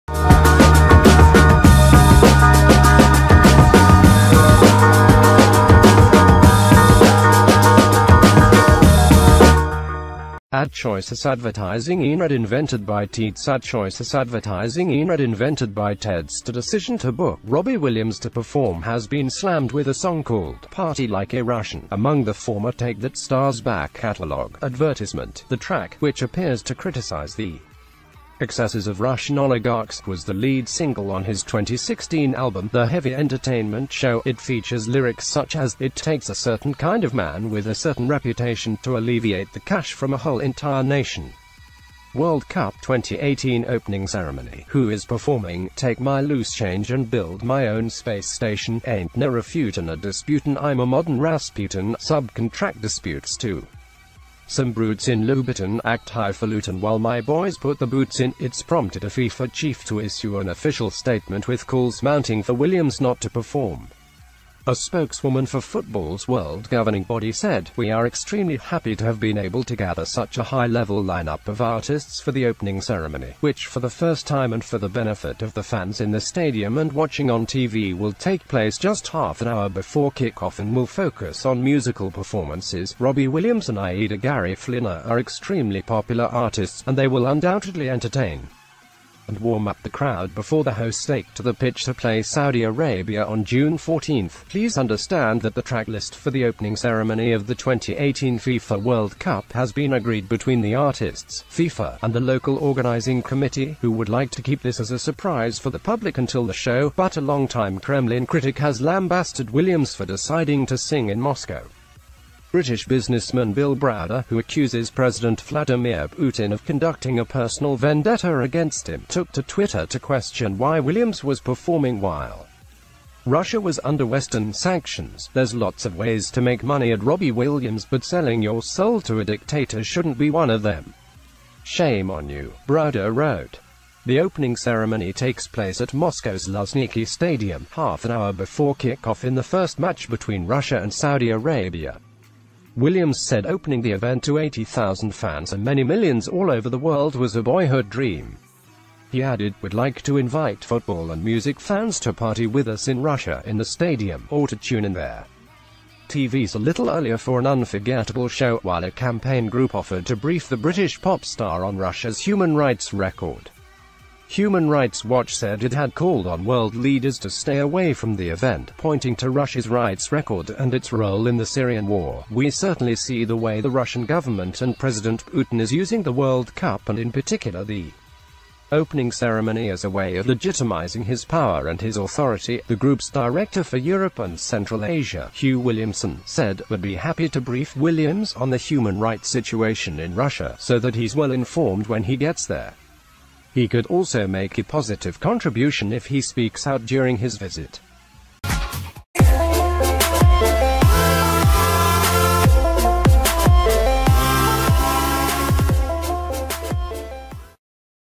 World Cup opening ceremony drama as Robbie Williams statement issued by FIFA amid chaos
► AdChoicesADVERTISINGinRead invented by TeadsAdChoicesADVERTISINGinRead invented by TeadsThe decision...